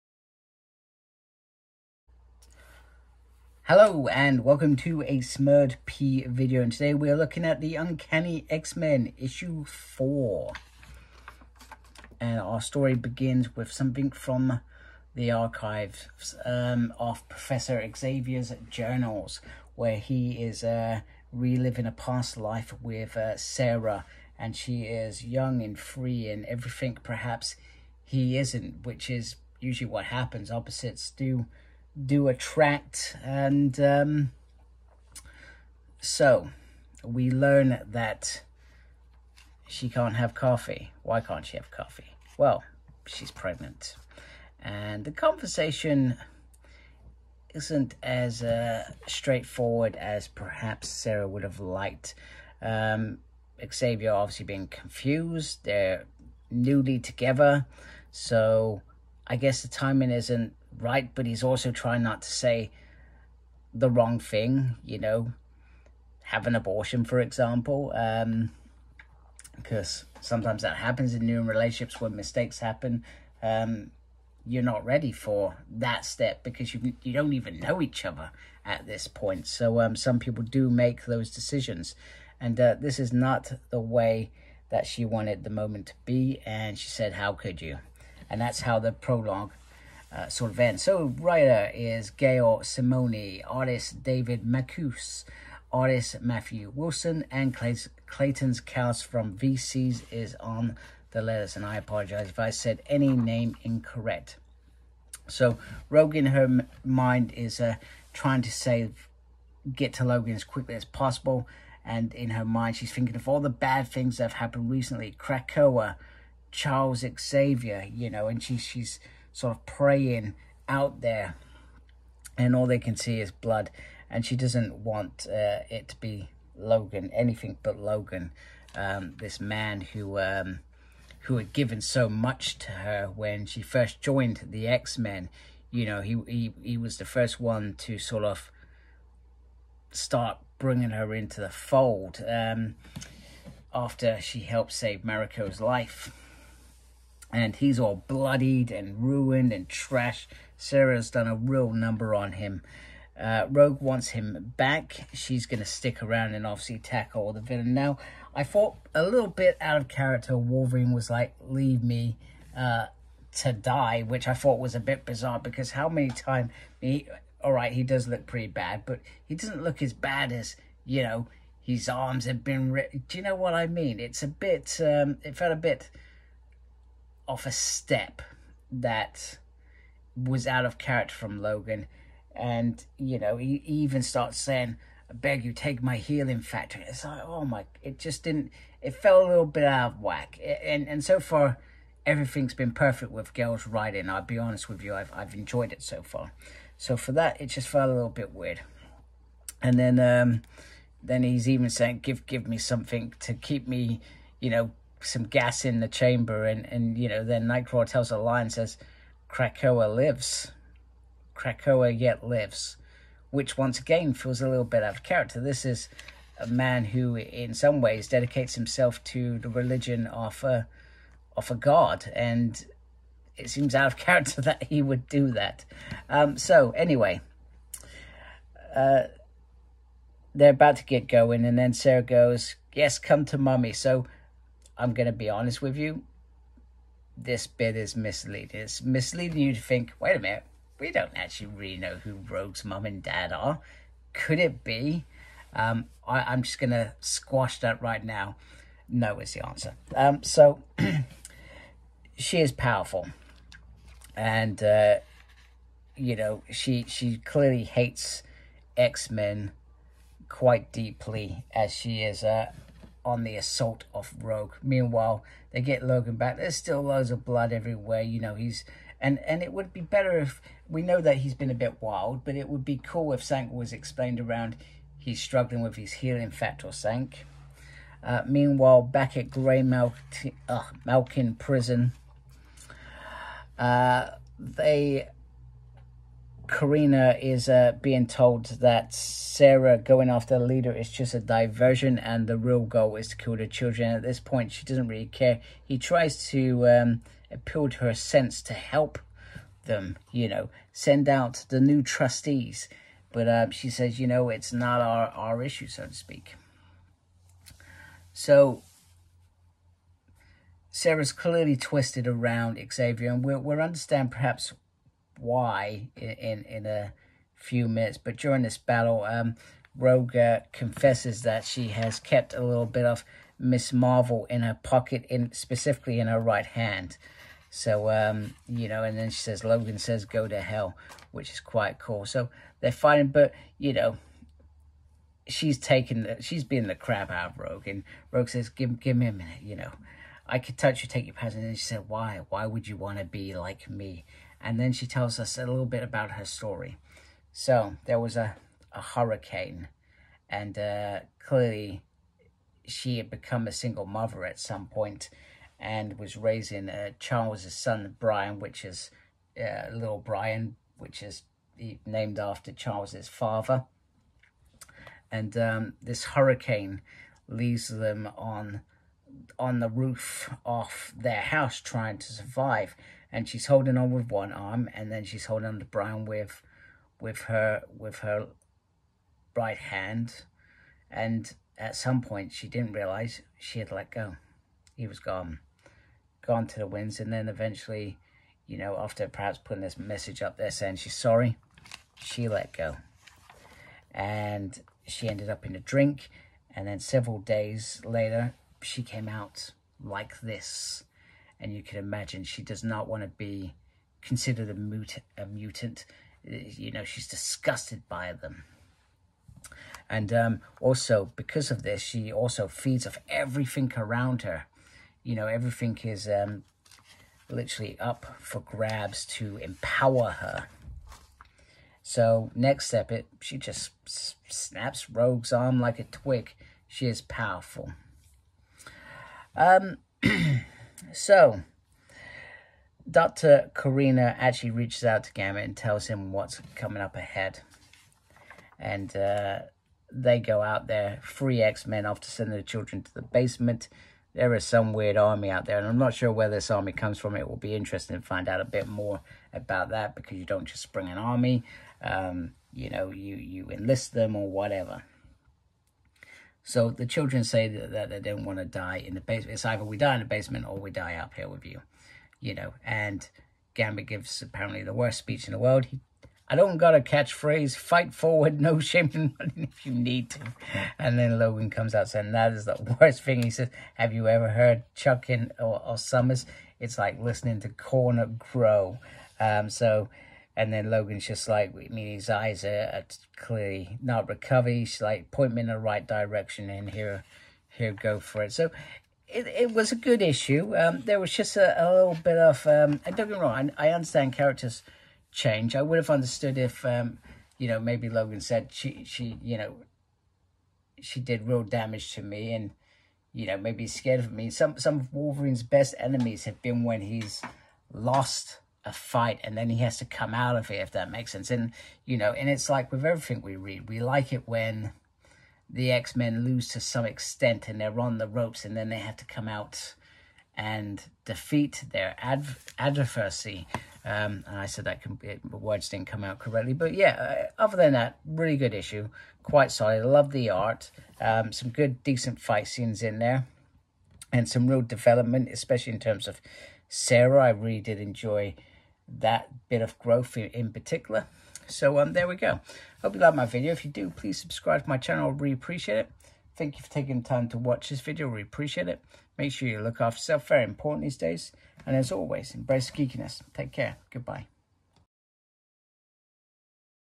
Uncanny X-Men #4 by Gail Simone | Marvel Comics Book Review | From The Ashes!
Uncanny X-Men #4
From: Uncanny X-Men
Universe: Marvel
Author: Gail Simone
Artist: David Marquez
Published by: Marvel Comics

Product Description
With one X-Man down and hell coming for the rest, ROGUE finds herself alone against a power of darkness she is completely unprepared for.
No backup, no lifeline and NO WAY OUT. And as she fights alone, a secret of the new recruits is revealed, is one of them the ENDLING that will destroy all mutantkind?

Embrace the Geekiness